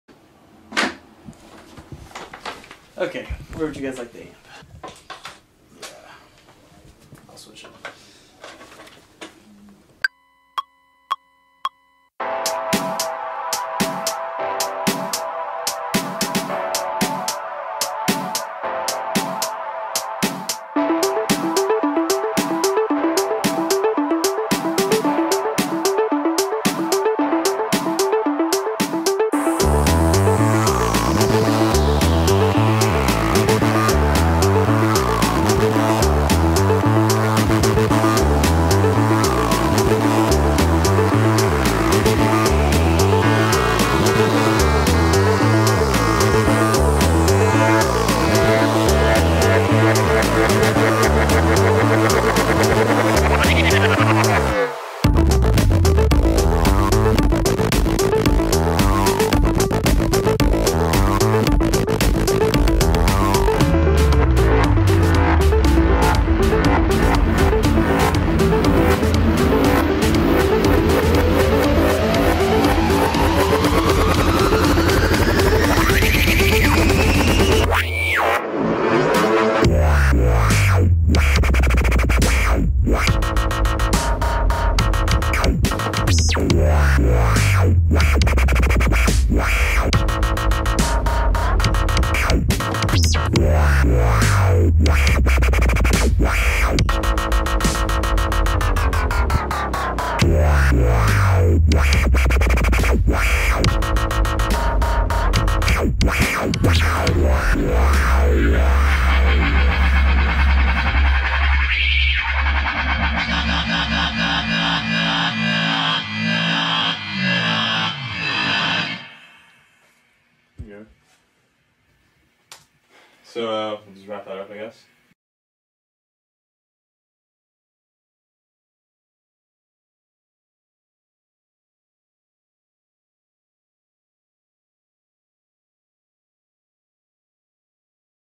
Guitar Wing Demo
This is a video demonstrating a new approach to "playing" electronic music. (ie. real-time synthesis.)
As music moves into the digital age, much of the Human element of performance has been lost.

The Roland GR-55 along with Livid Instruments' new product The Guitar Wing enables creative programming to be combined with the subtlety of human touch. Such programming includes the color changing, pressure sensitive pads and the built in accelerometer: which was programmed to the LFO rate.